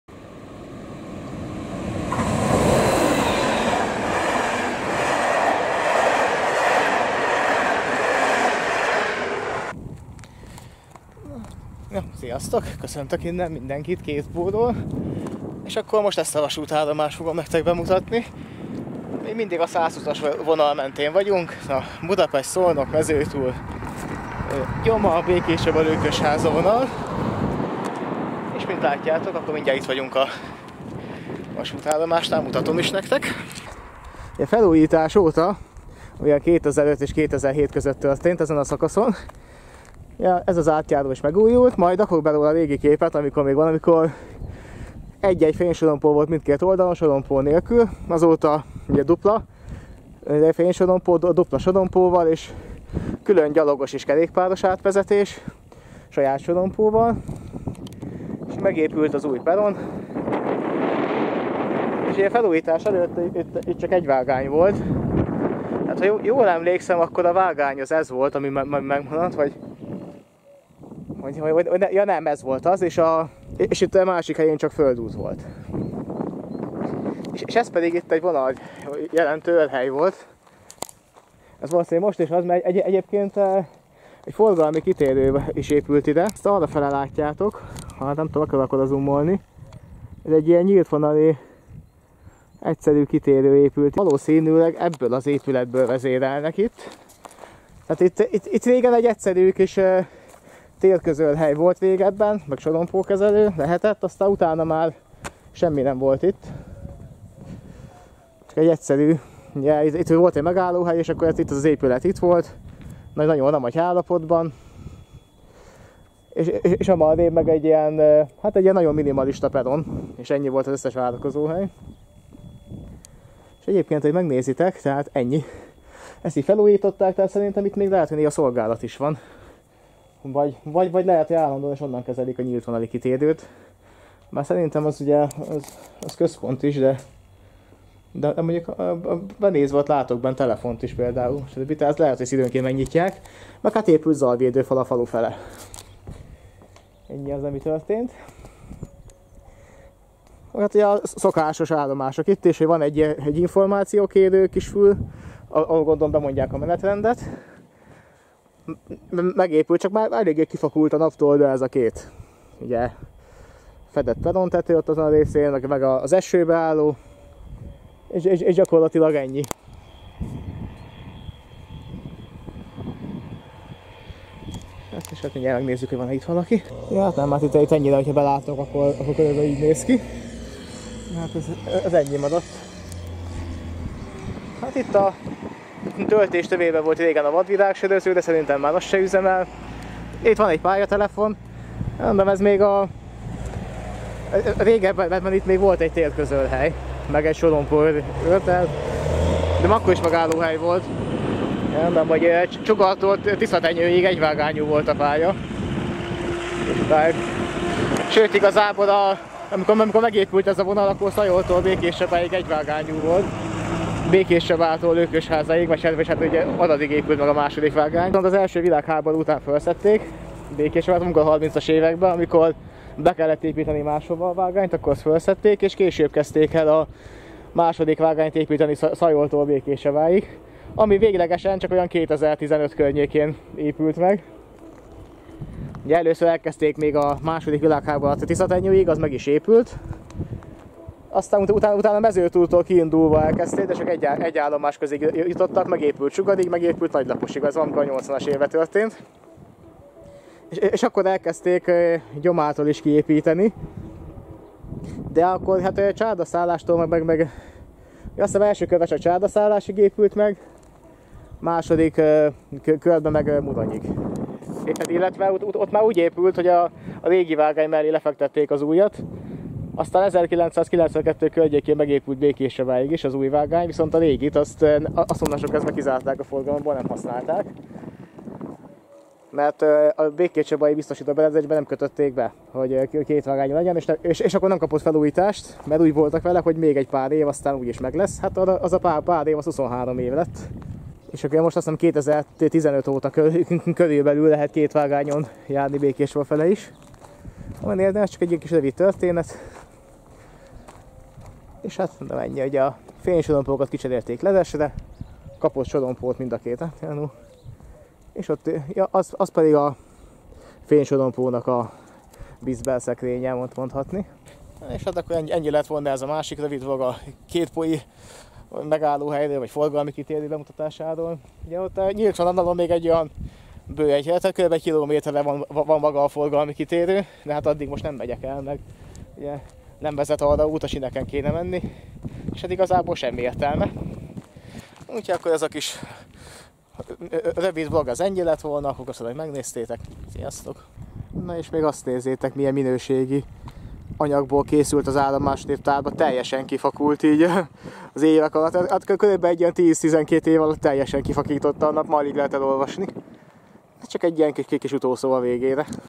2019.03.04 Vlog Kétpó a megállóhelyből lett forgalmi kitérő
Felújítás után megszűnt a két korábbi állomás Csugar és Pusztapó helyettük kb. félúton Kétpó egykori megállóhely mellett épült egy forgalmi kitérő.